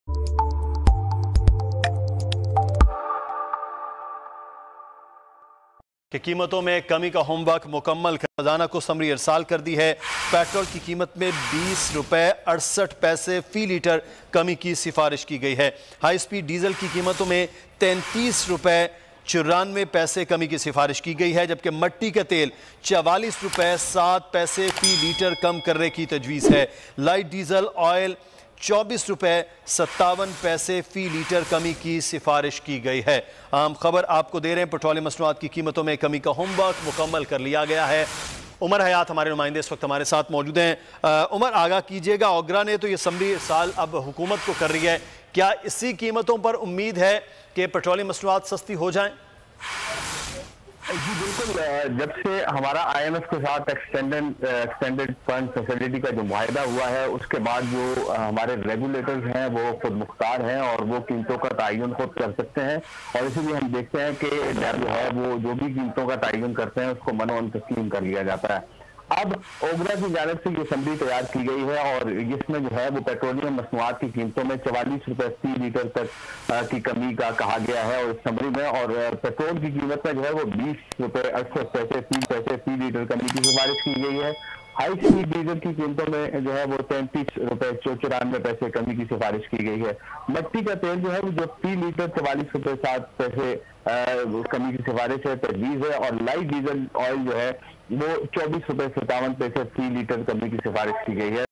OGRA Rrecommends Reduction in Petrol Prices | Pakistan Govt To Reduce Petrol Prices In May 2020
Petrol Prices Are Going To be Decreased In May 2020